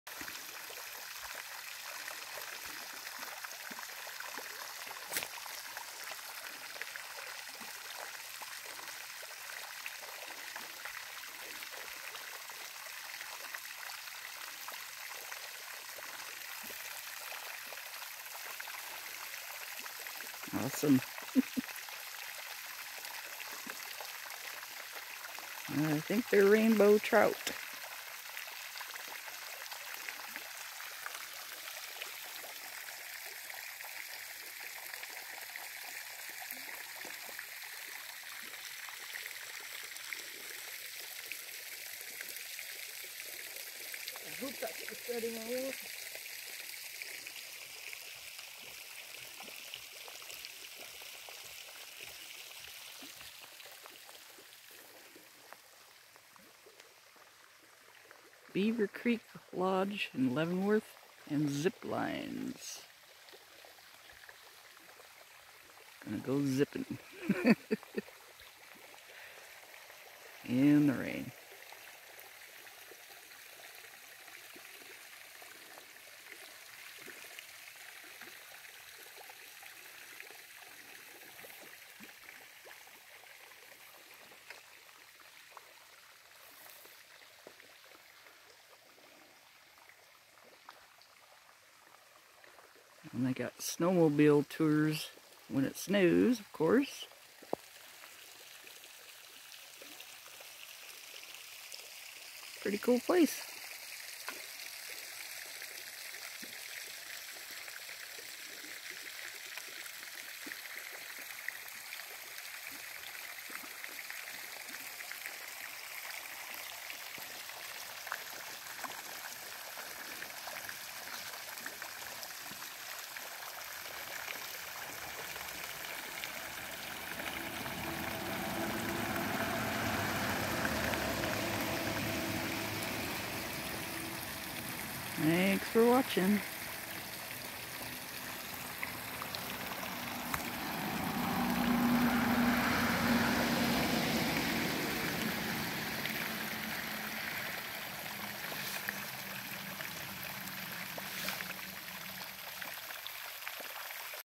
Fresh water trout pond
Huge trout at the Leavenworth Ziplines at Mountain Springs Lodge